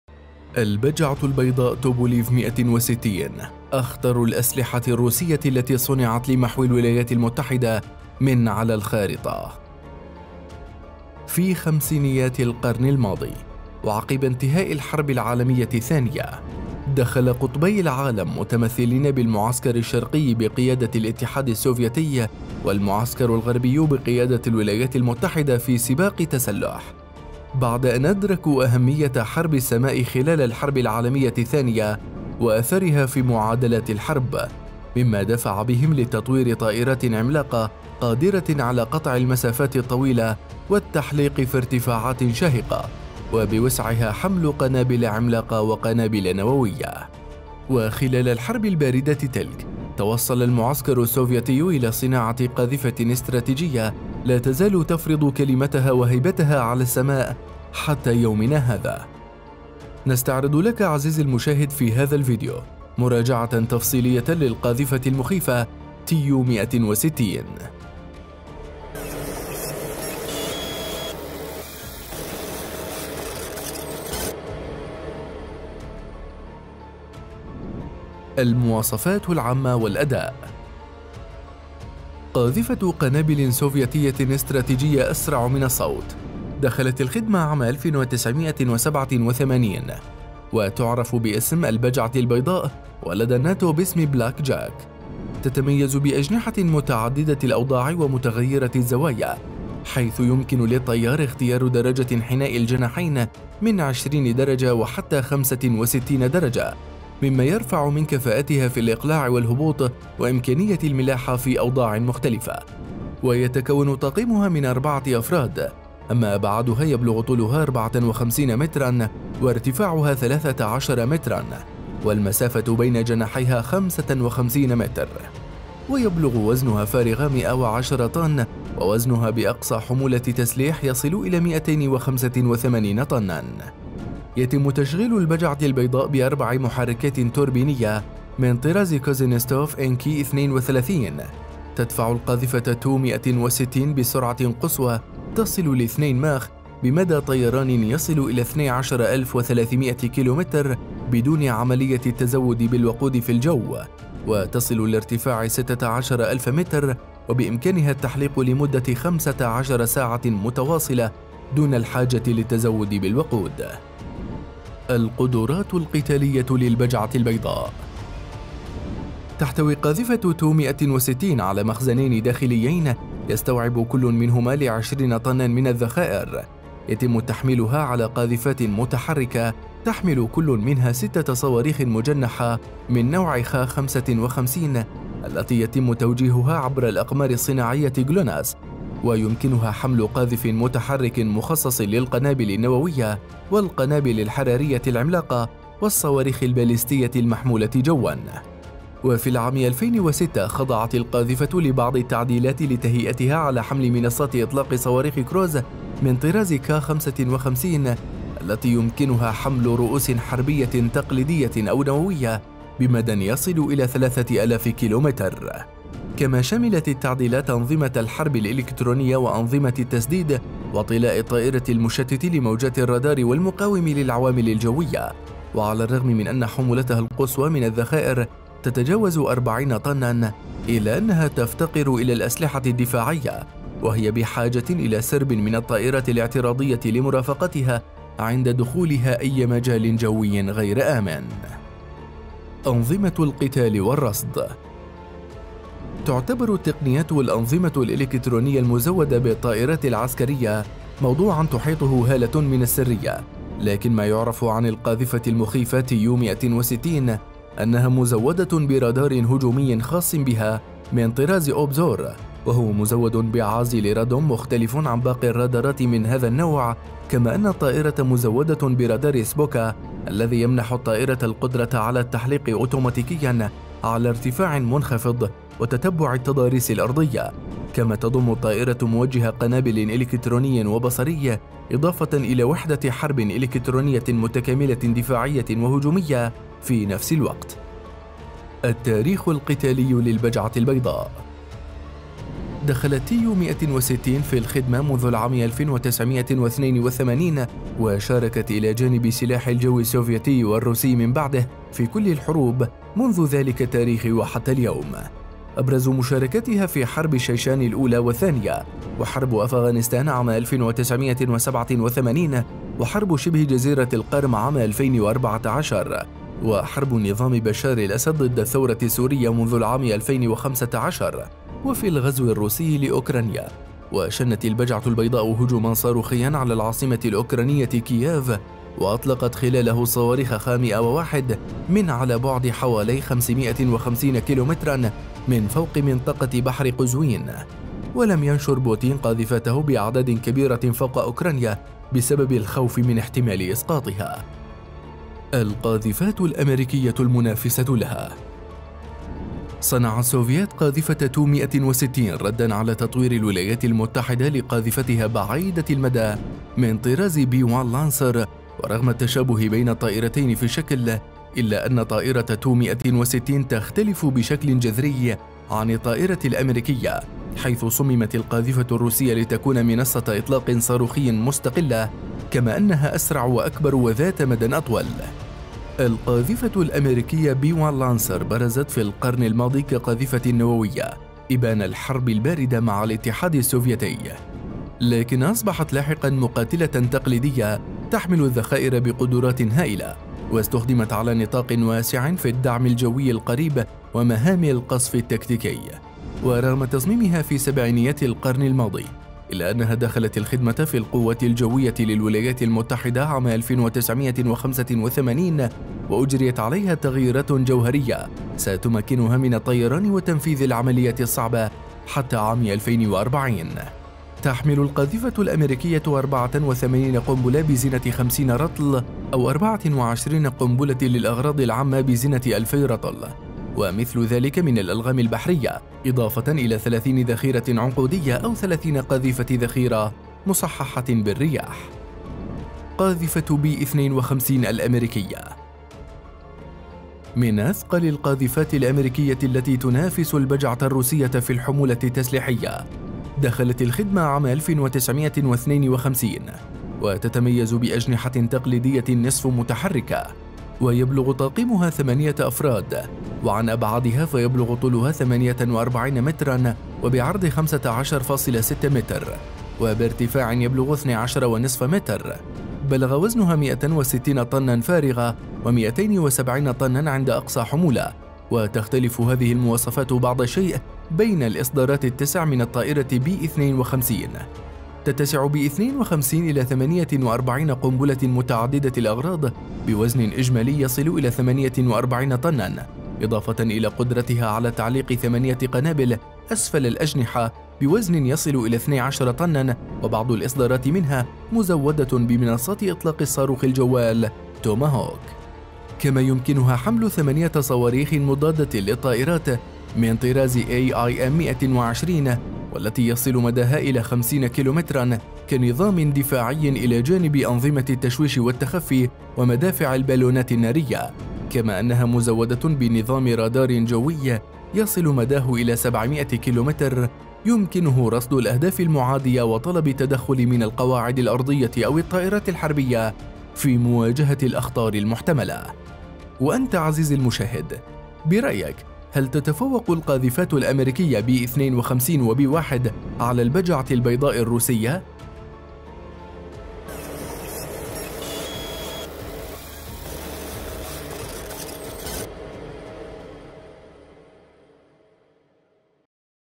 قاذفات مخيفة |حلقة 01| ..البجعة البيضاء .. اخطر الاسلحة الروسية التي صُنِعة لمحو امريكا
ملفات عسكرية | قاذفات مخيفة |حلقة 01| البجعة البيضاء توبوليف تو 160 .. اخطر الاسلحة الروسية التي صُنِعة لمحو الولايات المتحدة مع على الخارطة !!
White Swan Tupolev Tu-160 .. The most dangerous Russian weapon that was designed to terminate the United States from the map!!
في خمسينيات القرن الماضي، وعقب انتهاء الحرب العالمية الثانية، دخل قطبي العالم متمثلين بالمعسكر الشرقي بقيادة الاتحاد السوفيتي والمعسكر الغربي بقيادة الولايات المتحدة في سباق تسلح، بعد أن أدركوا أهمية حرب السماء خلال الحرب العالمية الثانية وأثرها في معادلات الحرب، مما دفع بهم لتطوير طائرات عملاقة قادرة على قطع المسافات الطويلة والتحليق في ارتفاعات شاهقة وبوسعها حمل قنابل عملاقة وقنابل نووية.

وخلال الحرب الباردة تلك، توصل المعسكر السوفييتي إلى صناعة قاذفة استراتيجية لا تزال تفرض كلمتها وهيبتها على السماء حتى يومنا هذا.

المواصفات العامة والأداء
قاذفة قنابل سوفيتية إستراتيجية أسرع من الصوت، دخلت الخدمة عام 1987 وتعرف باسم "البجعة البيضاء ولدى الناتو بإسم بلاك جاك .
0:00 - مقدمة
1:19 - المواصفات العامة والاداء
2:45 - القدرات القتالية للبجعة البيضاء
4:02 - انظمة القتال والرصد
4:48 - التاريخ القتالي للبجعة البيضاء
7:13 - قاذفة بي 52
8:47 - النهاية
البجعة البيضاء
توبوليف تو 160
بلاك جاك

تابع قناة ملفات عسكرية على Facebook